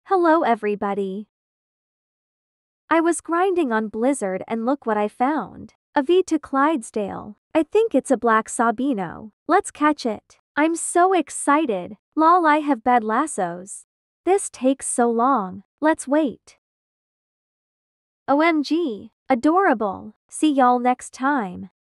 Catching my SECOND V2 Clydesdale
hello everyone! ~{orchid}~ here

This short video features me catching my second V2 Clydesdale!

This is my first time using the robot/computer voices on CAPCUT!

This voice is called: Energetic Female